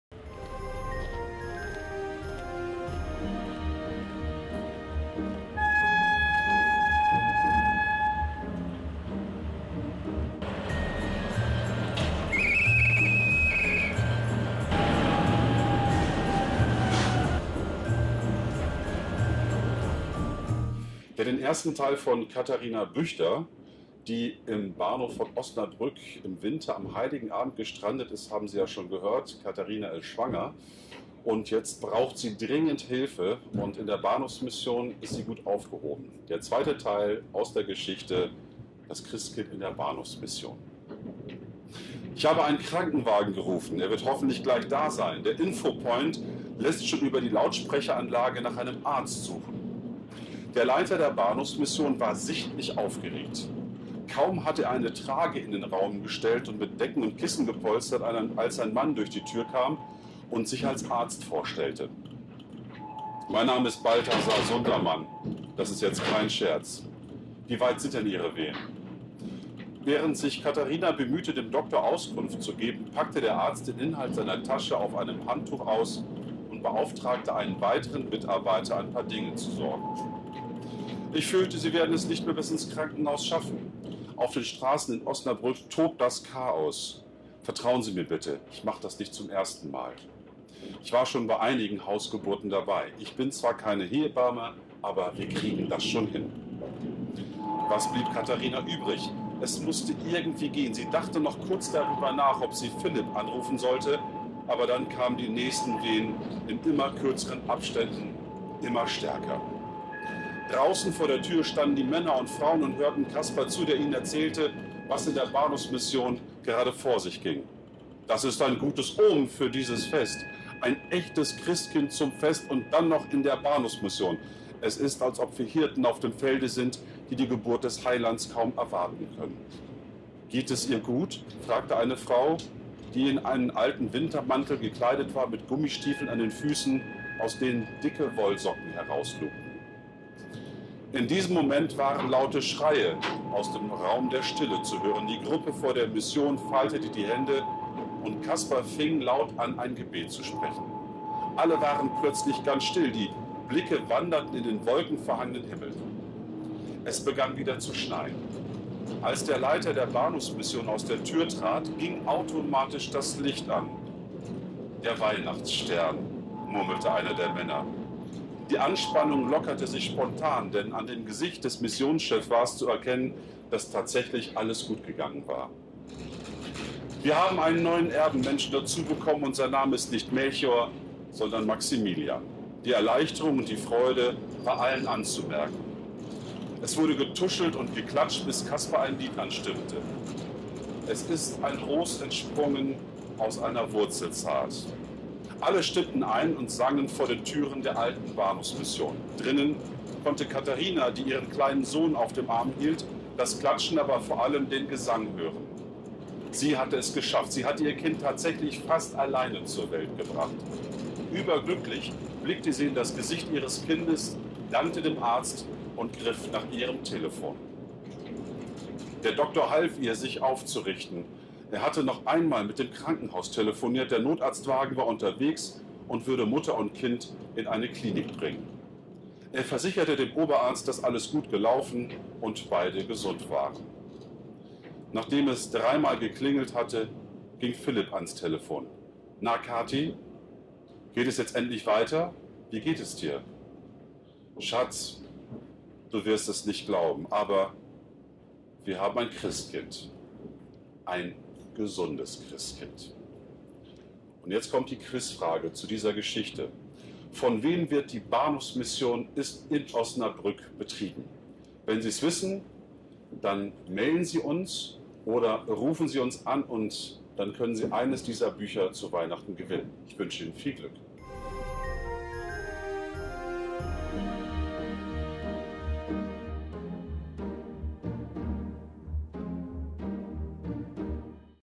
Weihnachten im Zug Teil 2
Der Adventsengel 👼wünscht Euch einen besinnlichen 2. Advent 🕯️🕯️
Heute liest Ludger Abeln den zweiten Teil der Geschichte "Das Christkind in der Bahnhofsmission" vor (Teil 1 findet Ihr
Heute könnt Ihr ein handsigniertes Exemplar von Ludger Abelns Buch "Weihnachten im Watt" gewinnen.
Viel Erfolg!